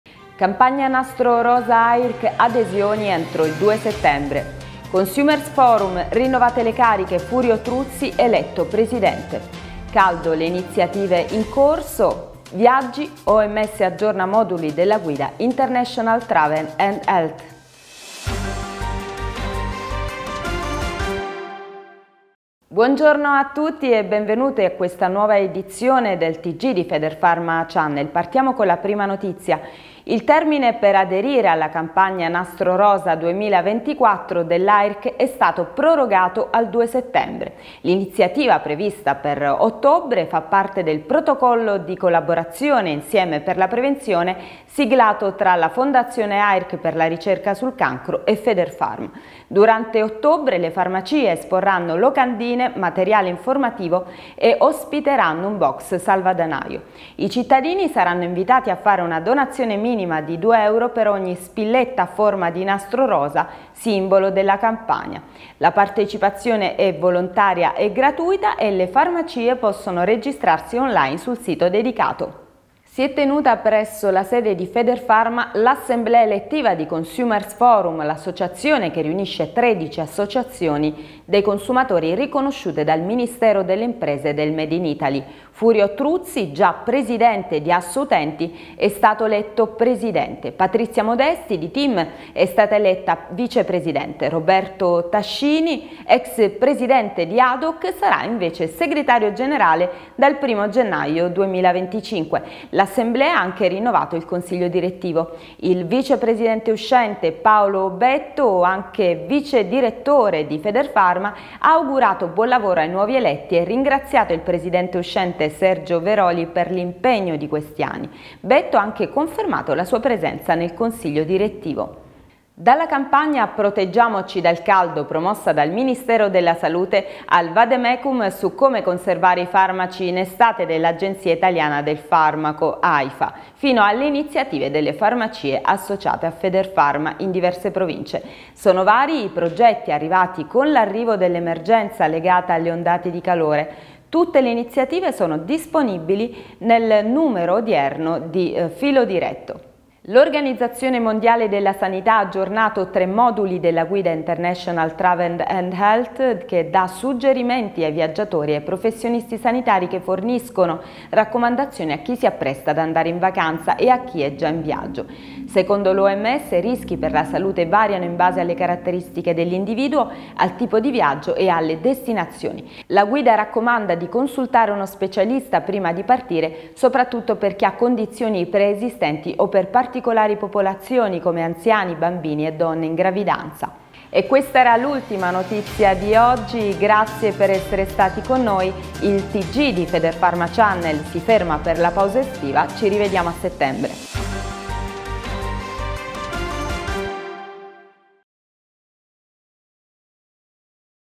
Tg Federfarma Channel - Edizione dell'1 agosto
In questo numero: Campagna Nastro Rosa Airc, adesioni entro il 2 settembre; Consumers’ forum, rinnovate le cariche: Furio Truzzi eletto presidente; Caldo, le iniziative in corso; Viaggi, Oms aggiorna moduli della guida “International travel and health”.